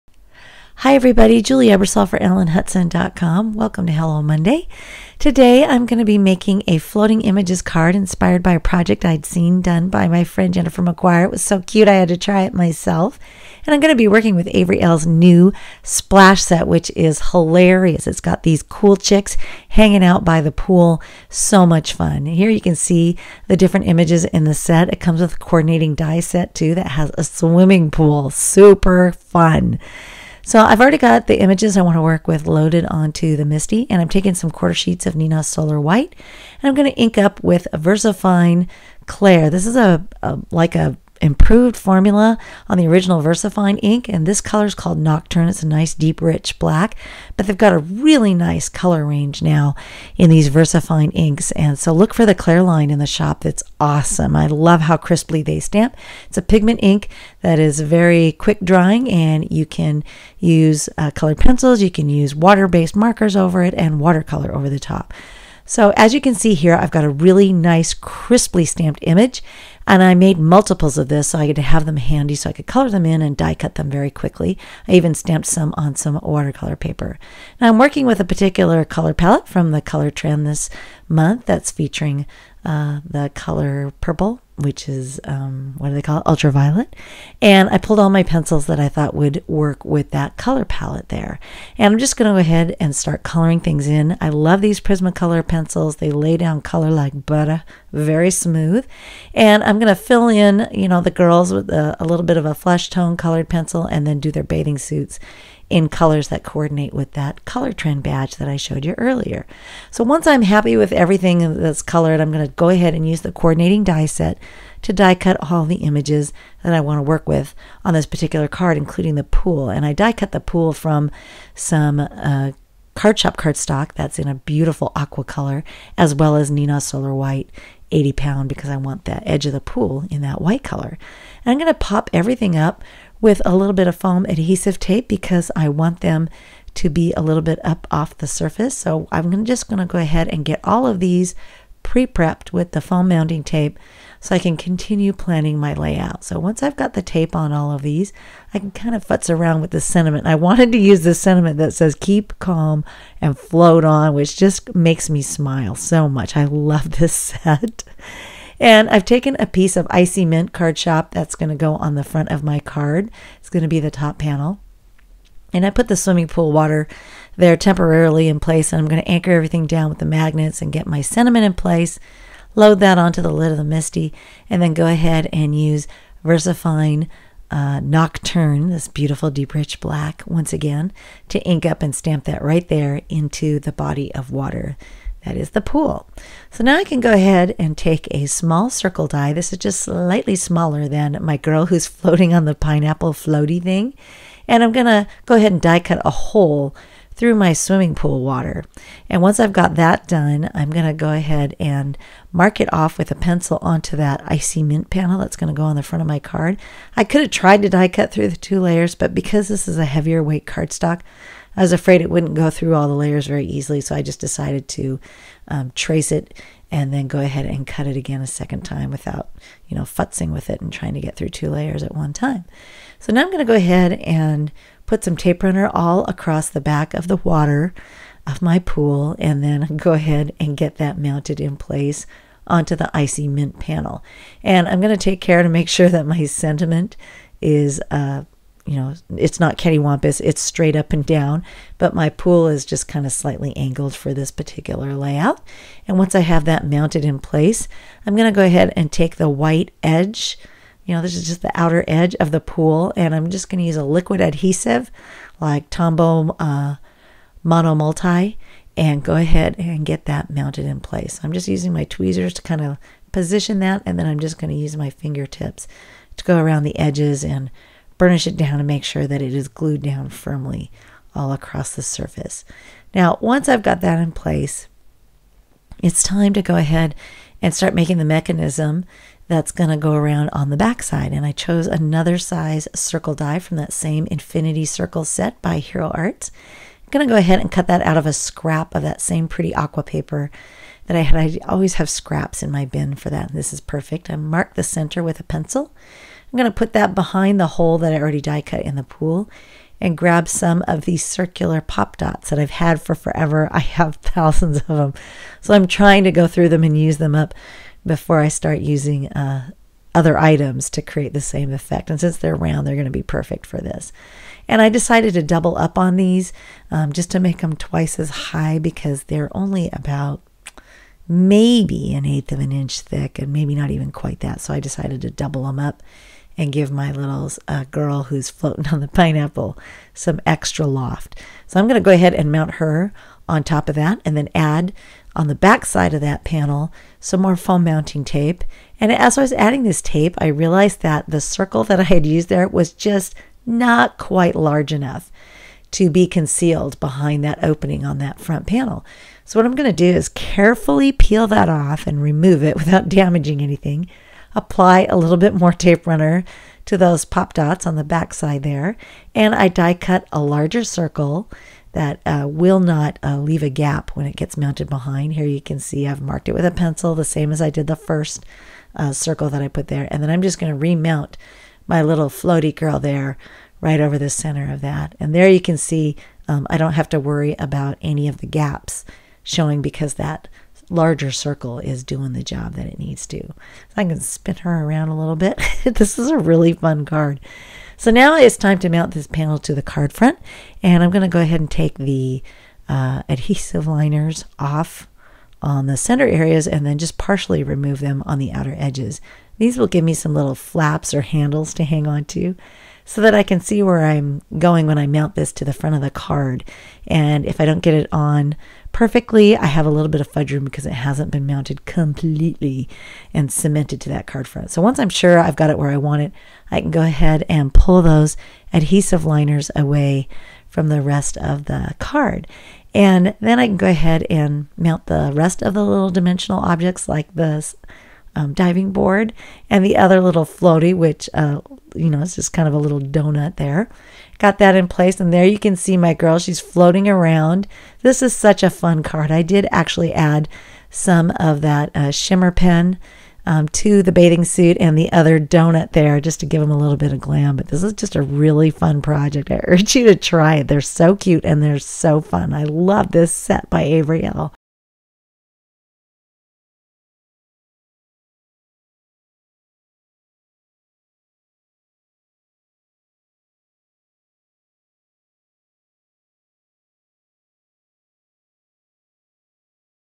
Floating Images Interactive Card - Hello, Monday 02/12/2018
How to make your images "float" on the front of your card!  Inspired by Jennifer McGuire,
Supplies List:
Avery Elle Clear Stamps, Splash!
My Sweet Petunia Mini MISTI Laser Etched Stamping Tool (6-1/8 X 7)